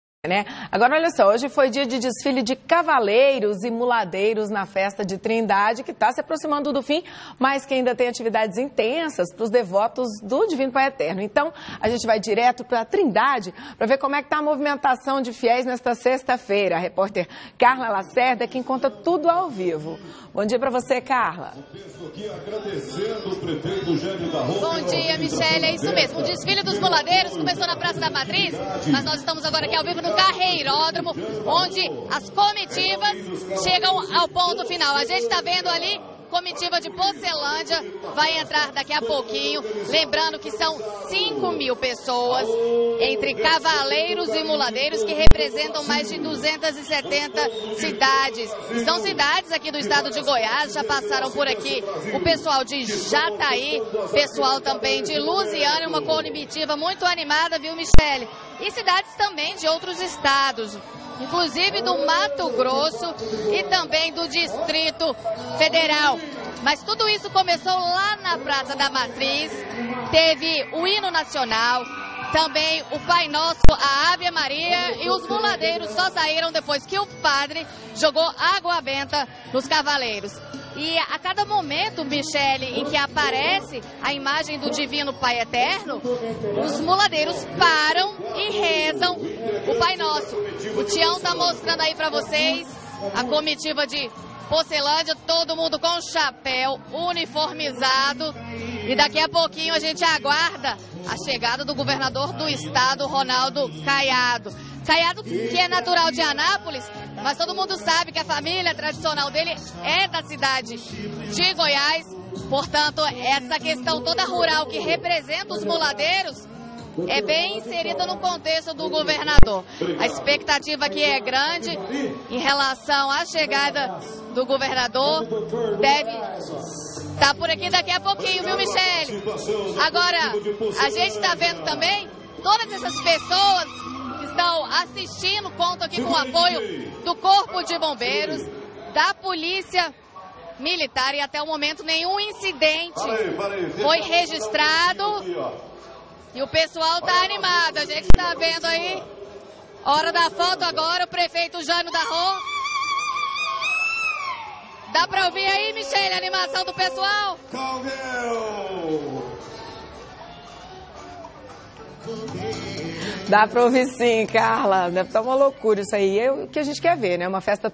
TBC 1 | DESFILE DE CAVALEIROS E MULADEIROS NA FESTA DE TRINDADE
Data de Publicação: 05/07/2019
Repórter: Carla Lacerda